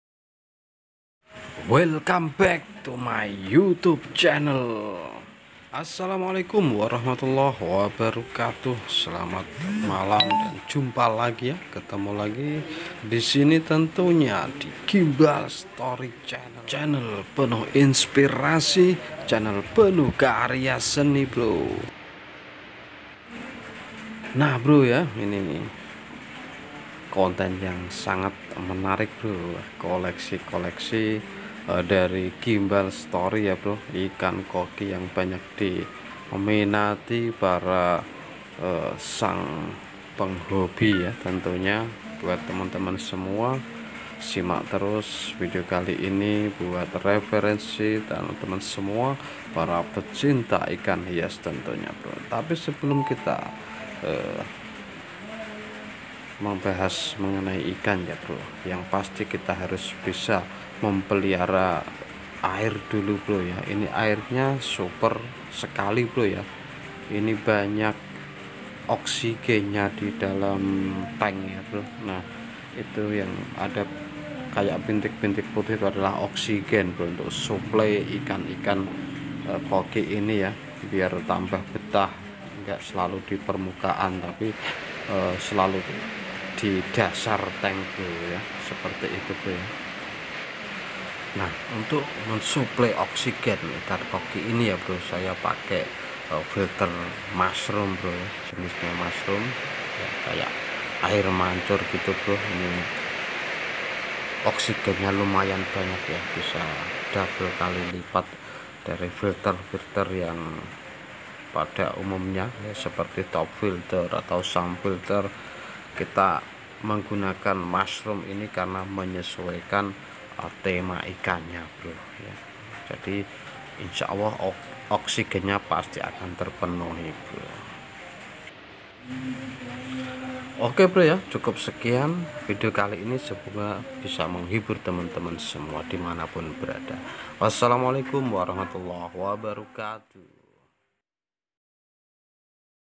Koleksi ikan koki gimbal story
Koleksi ikan koki gimbal story hanya untuk menghilangkan kepenatan demi ikan koki ini saya sangat betah di rumah untuk menyalurkan hoby dengan mengkoleksi ikan koki ini yang banyak di minati di pasaran khususnya di indonesia karena banyak juga penghoby ikan koki ini seperti koleksi ikan kokinya gimbal story.